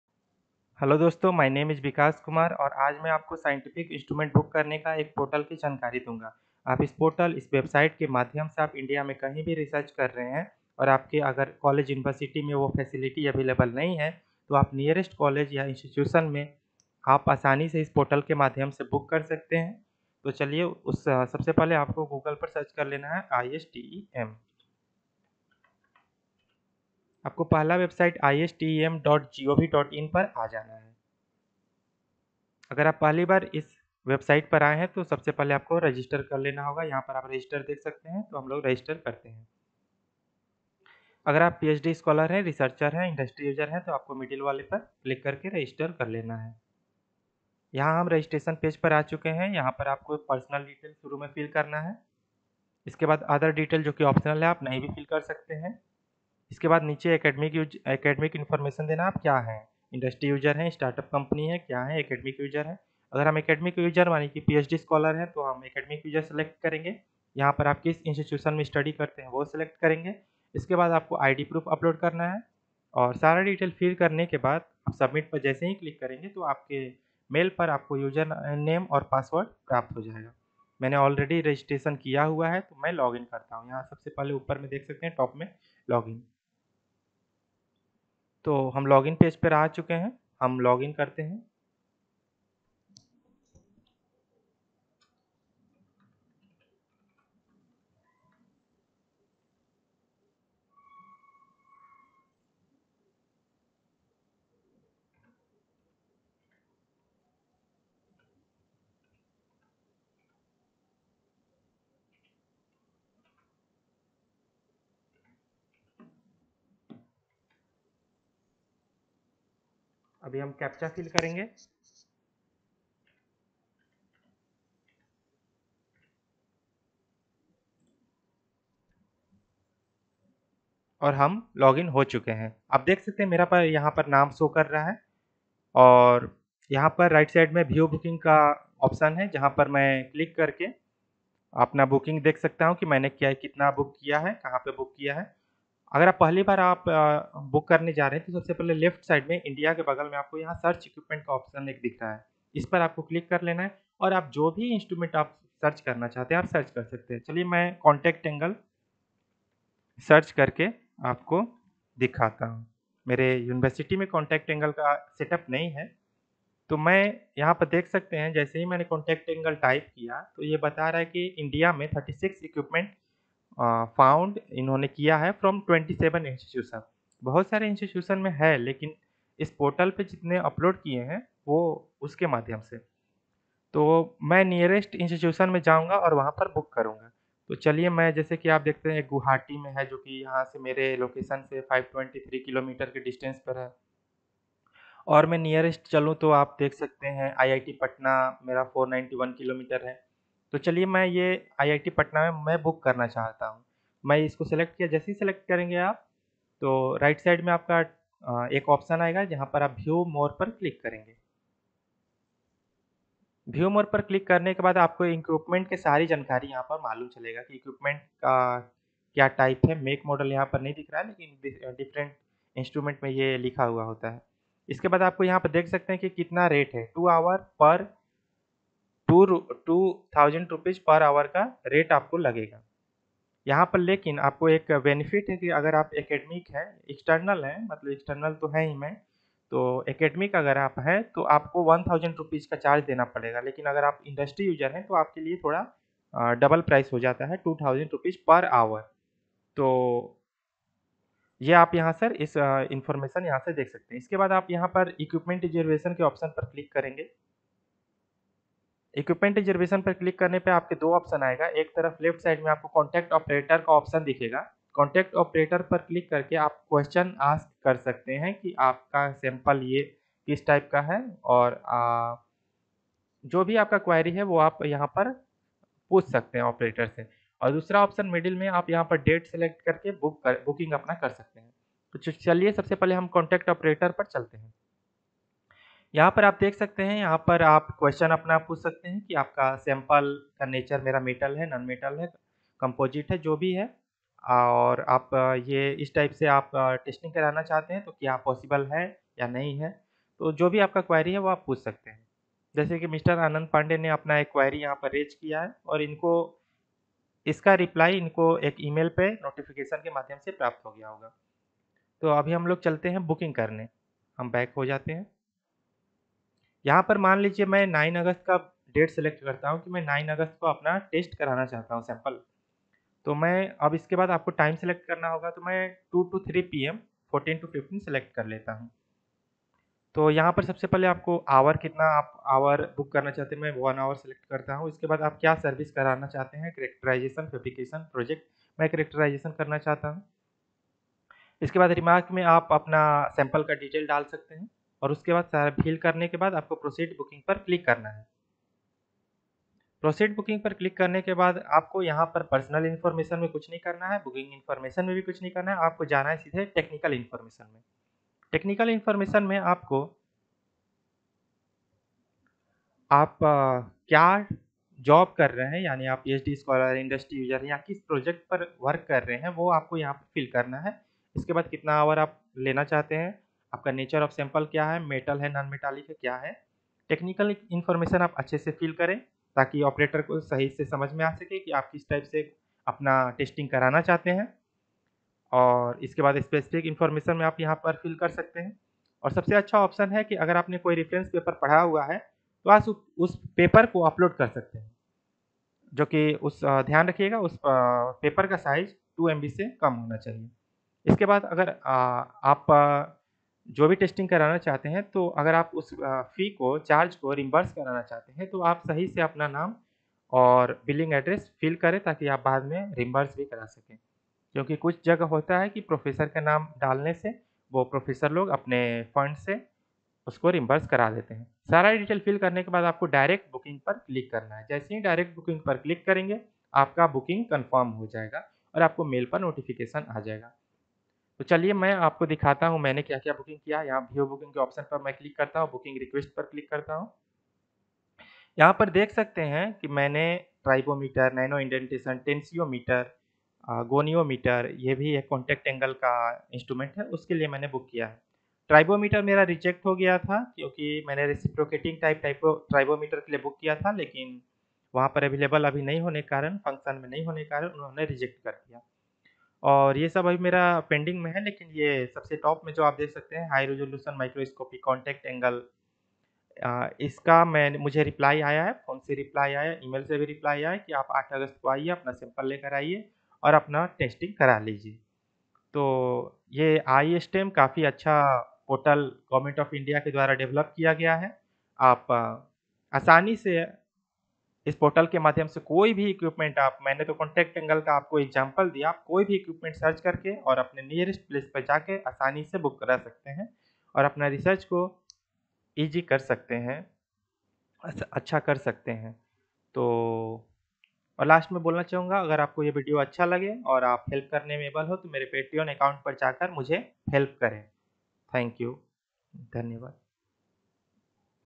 Book any scientific instrument for testing facility from one portal
It is a portal developed by Government of India where all the institutes share their tools which can be used by any user by online booking. A National Portal that is the gateway for researchers to locate the specific facility(ies) they need for their R&D work and identify the one that is either located closest to them or available the soonest.